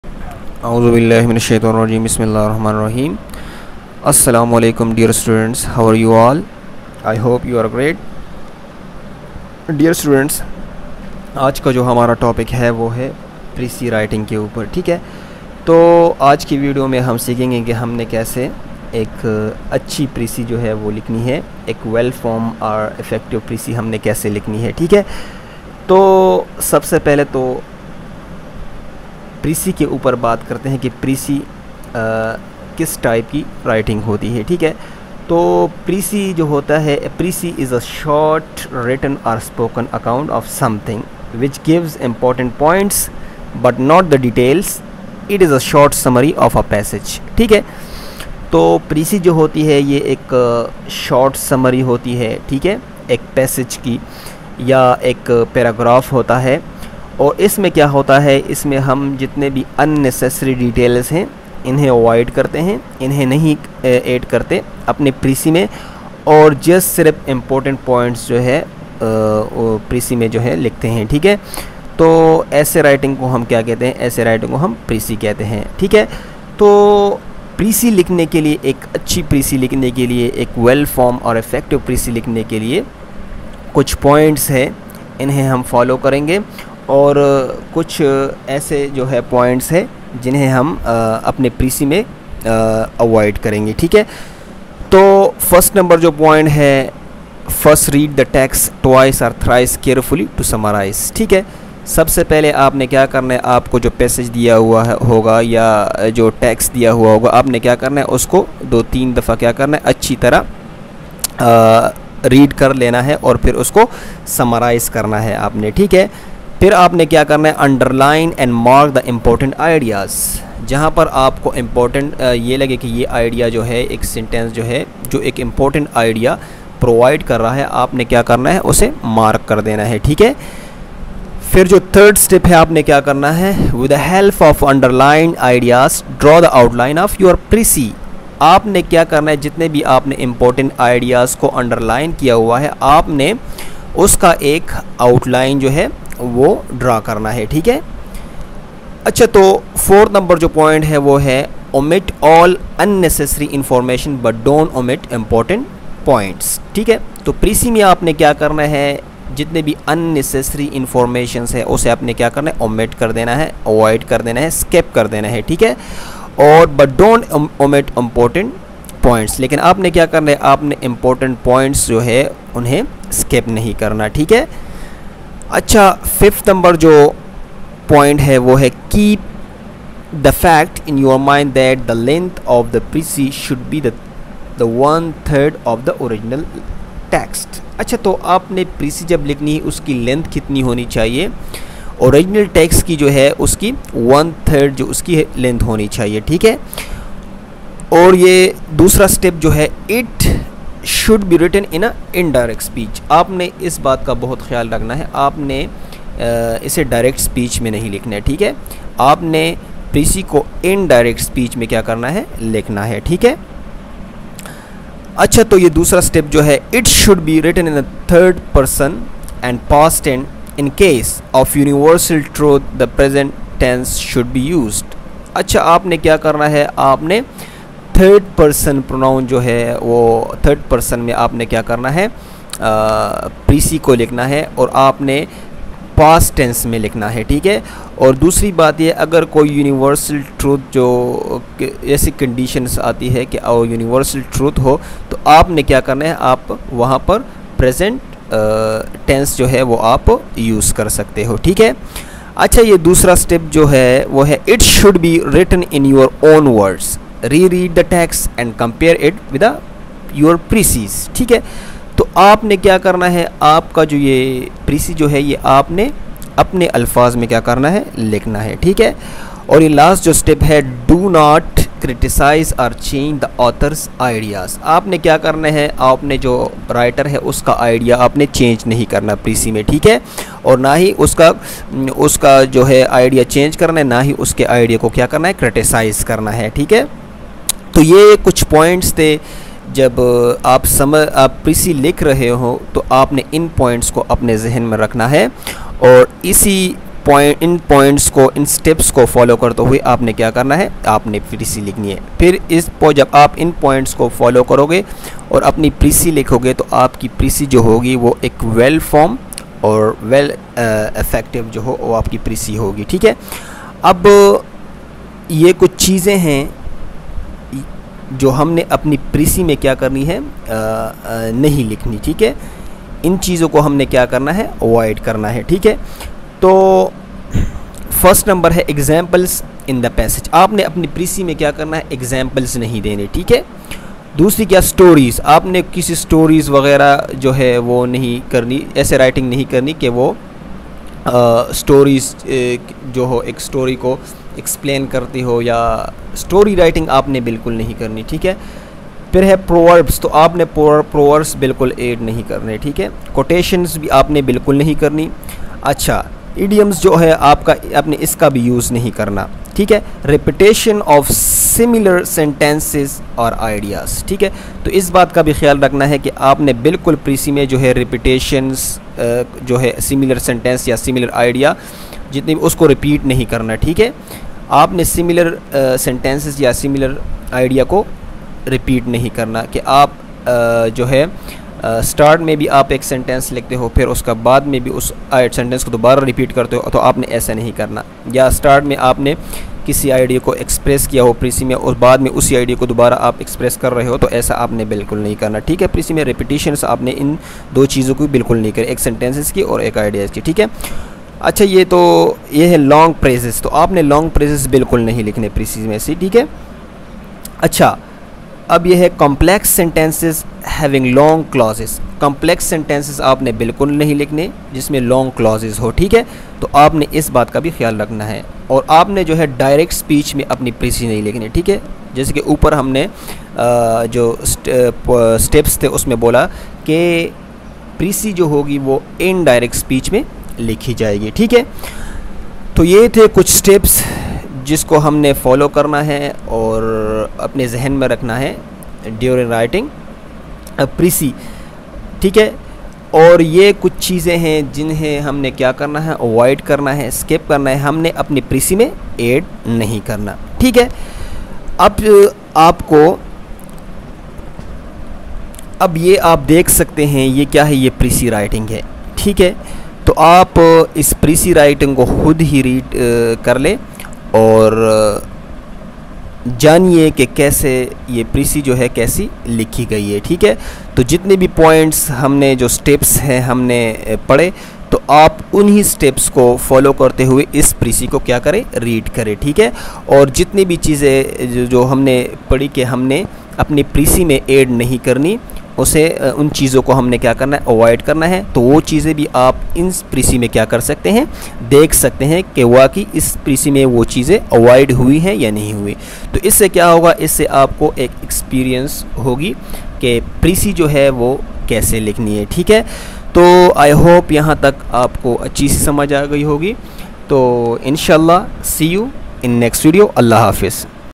Precis writing for css
Asslamulaikum,

I have mentioned some important steps about how to write a well form and effective precis .

this video will give you the way to write and be able to perceive a precis .